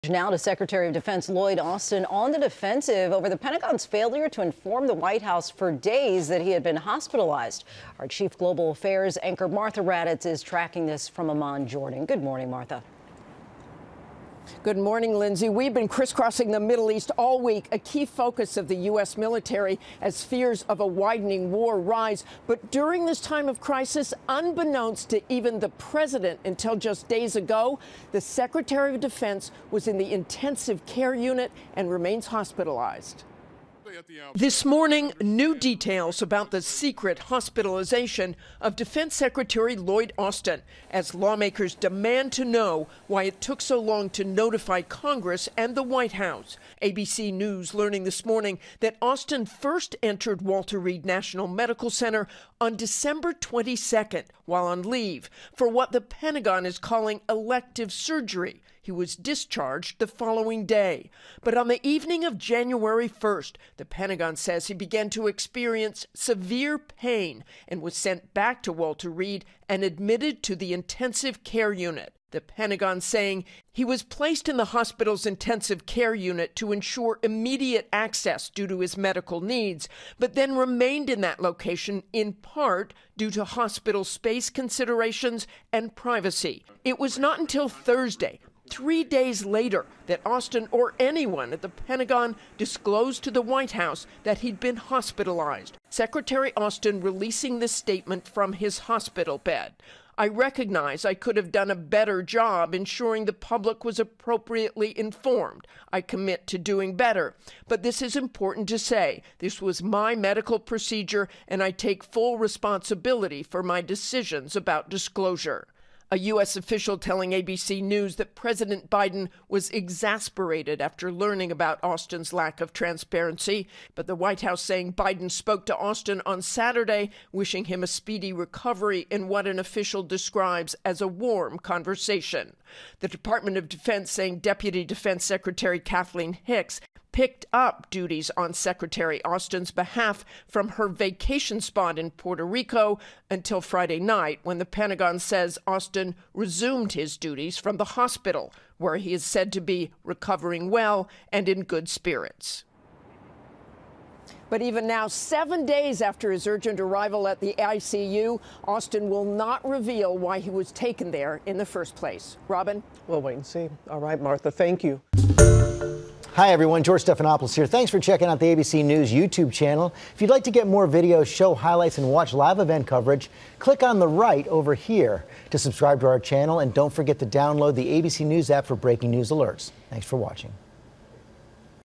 Defense secretary under fire for keeping White House and Congress in the dark over hospitalization
Sec. Lloyd Austin said he is taking "full responsibility" for keeping the White House in the dark about his hospitalization after complications from what was described as a minor medical procedure.